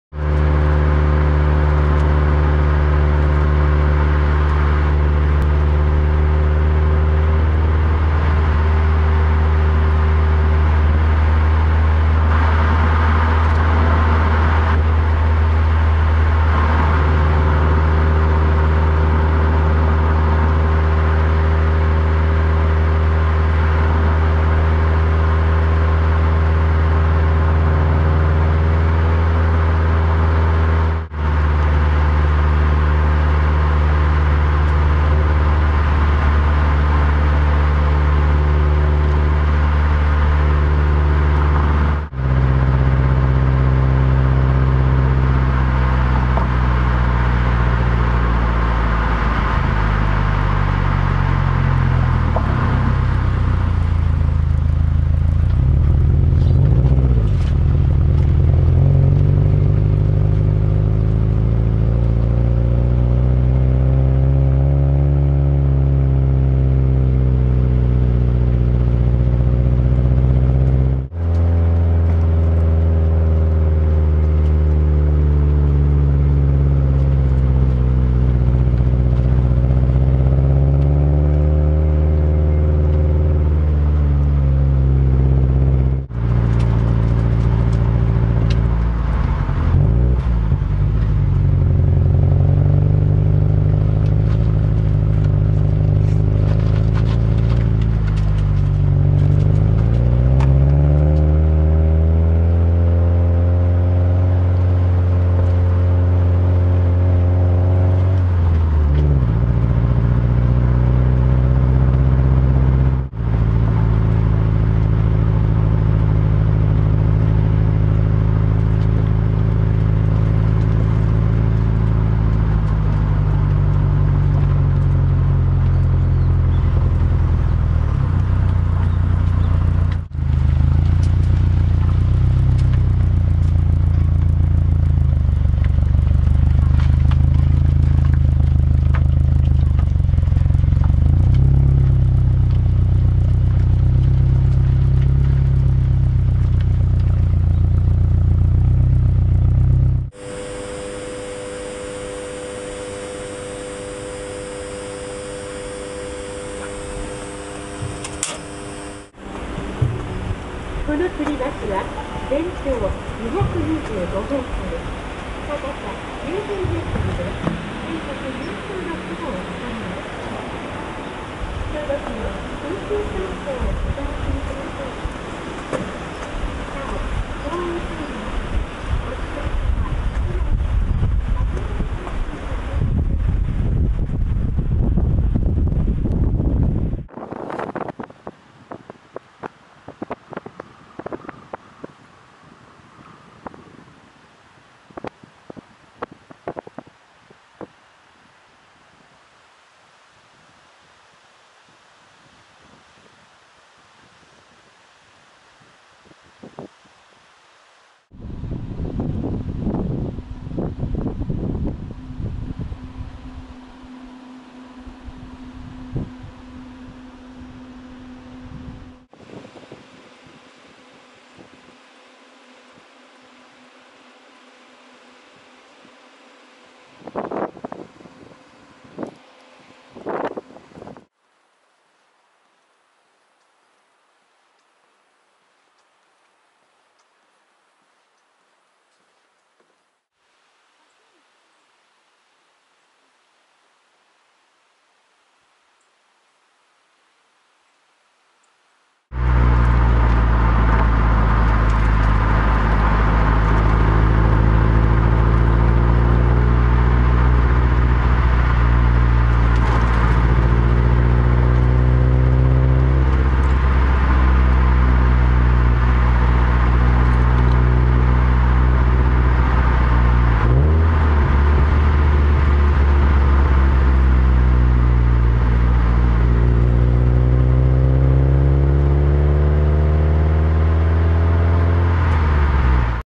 群馬県多野郡上野村 上野スカイブリッジへ 車載＆徒歩 WRX STI
国道299号線で志賀坂峠を越えて群馬県まで来たので、
上野村にある上野スカイブリッジまで行ってみました。

動画は車載動画と上野スカイブリッジの様子（徒歩）が半分ずつです。

0:00　上野スカイブリッジへ（車載動画）
2:30　橋の様子（徒歩）
4:10　帰路（車載動画）

次回は県道71号線で埼玉県側に帰ります。

車：WRX STI TypeS(VAB-A)

マイク：Audio-Technica AT9920（ステレオ・車外）

カメラ：SONY FDR-X3000

動画中の地図はOpenStreetMapを使用しています。